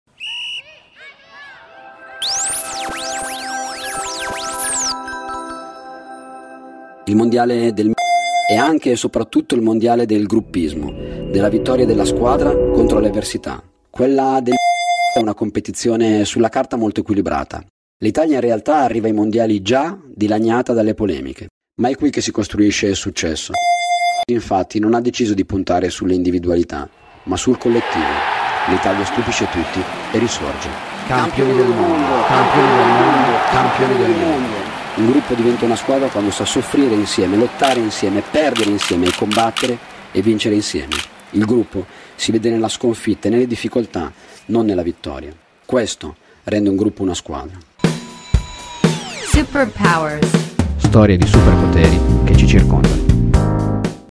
video trailerpodcast Superpowers - episodio 7
Video animato disegnato e ralizzato per raccontare in breve sui social la puntata n. 7 del podcast "Superpowers" di BIP.
In collaborazione con la content factory di BIP, disegni e animazioni di Laura Caleca, sceneggiatura di Simona Calo.
Speaker: Luca Martignon, Equity Partner di BIP.